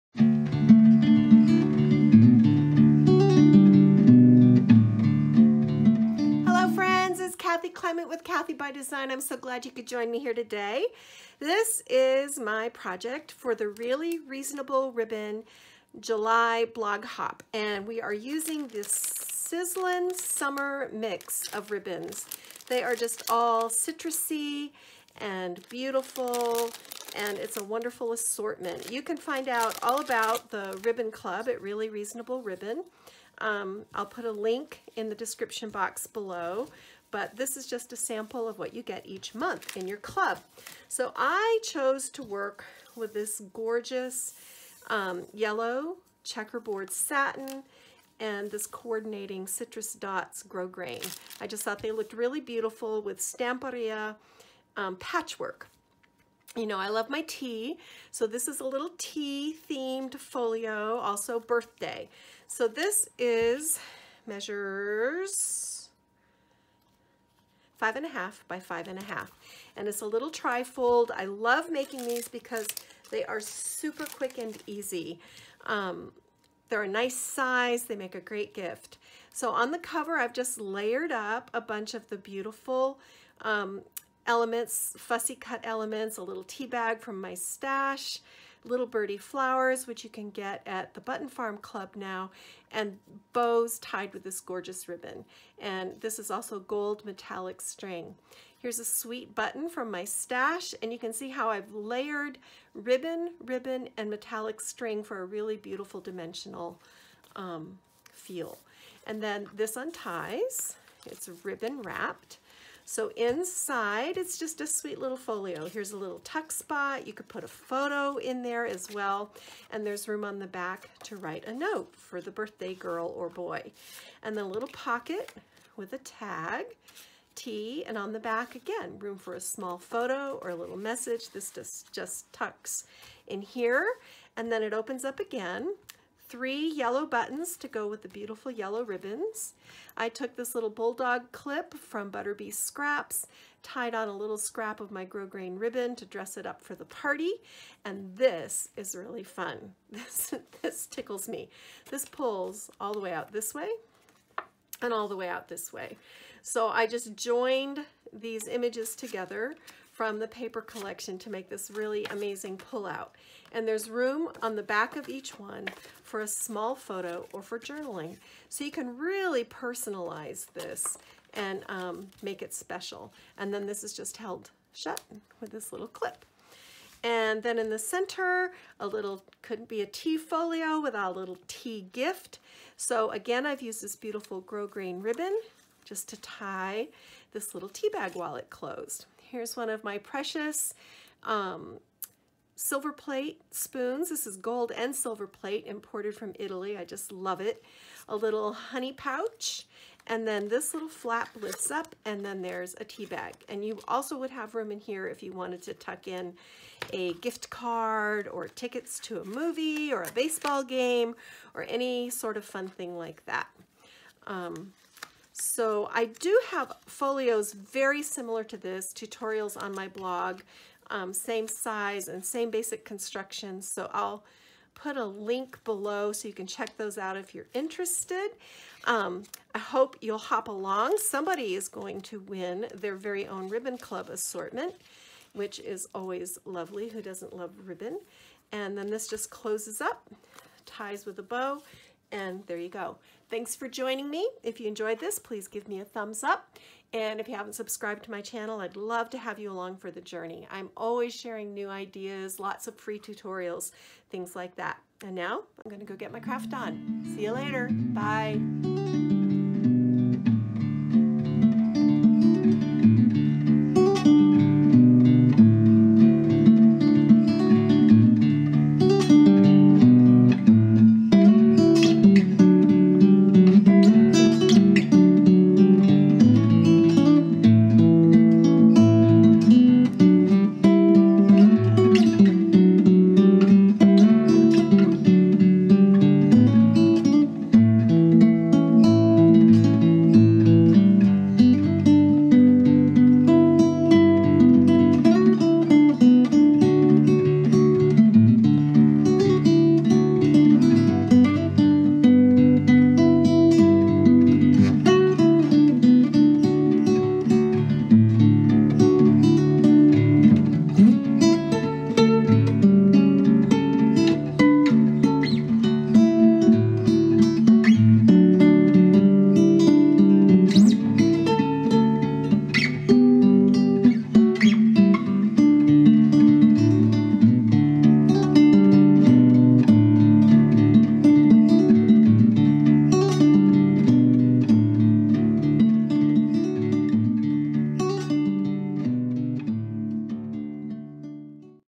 Stamperia Patchwork Special Day Tea Folio Project Share
Love tea?  Love folios?  Then join me for a quick project share of this Stamperia Patchwork "Special Day" Tea Themed folio.

I've created it especially for the Really Reasonable Ribbon July Blog Hop using the Sizzlin' Summer Ribbon Club Assortment.

  You'll find lots of ideas and tips that you can use in your own crafty adventures.

To find out how to make this 5 1/2" trifold folio base, watch this video tutorial on my channel: